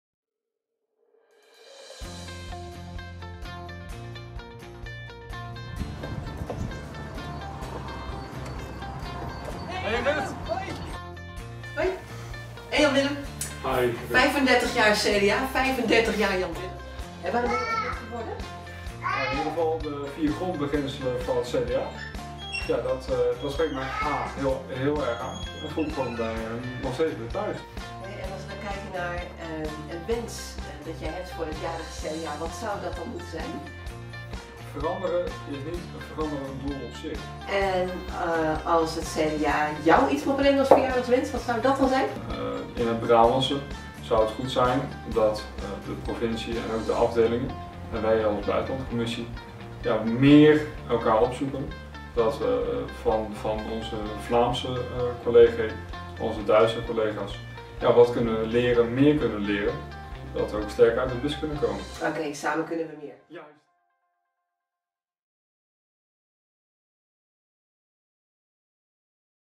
35 jaar CDA, op bezoek bij Jan-Willem Vogels
Het CDA bestaat 35 jaar. Mede daarom ging Ruth Peetoom op bezoek Jan-Willem Vogels, CDA-lid en eveneens 35 jaar!